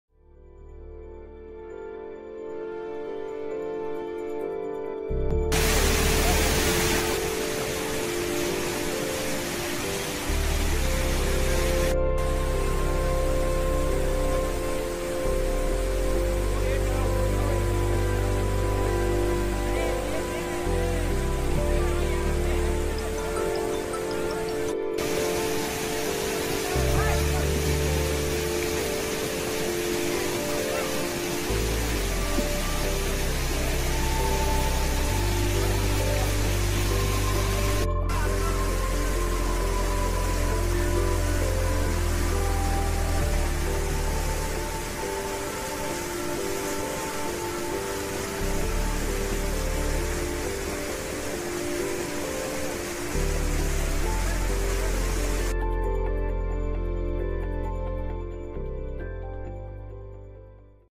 SDRF team rescues villagers in Uttarakhand's Vikasnagar
A team of State Disaster Response Force (SDRF) rescued over 20-30 villagers by helping them crossing a raging river.